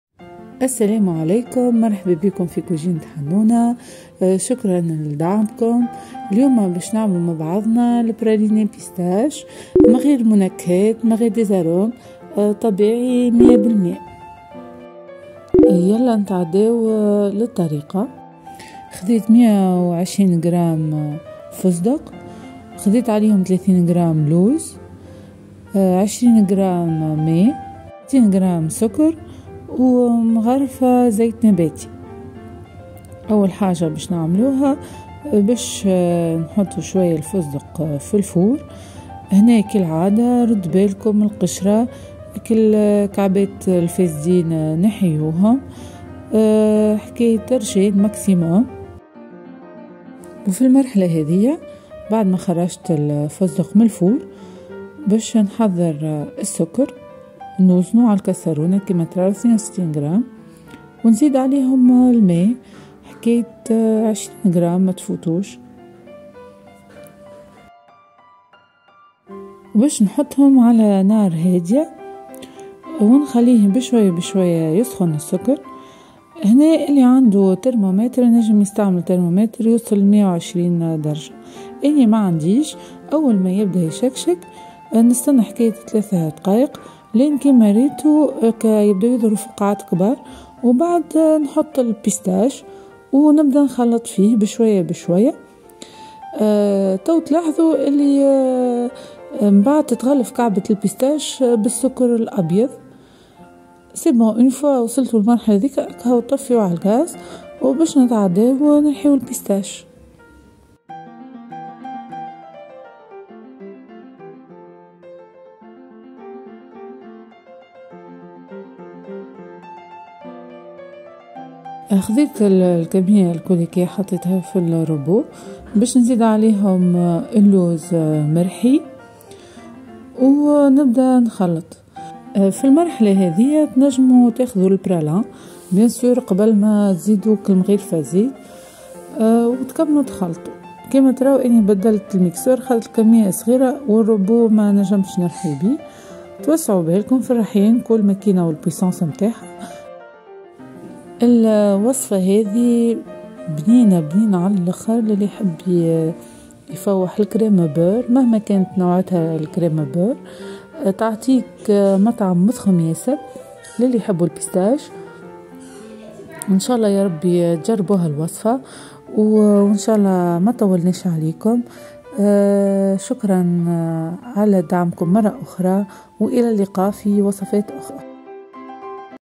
PRALINÉ PISTACHES 💯 وصفة ناجحة sans thermomètre alimentaire
الوصفة ؛ 120غ فزدق
30 غ لوز
60غ سكر
20 غ ماء
مغرفة زيت نباتي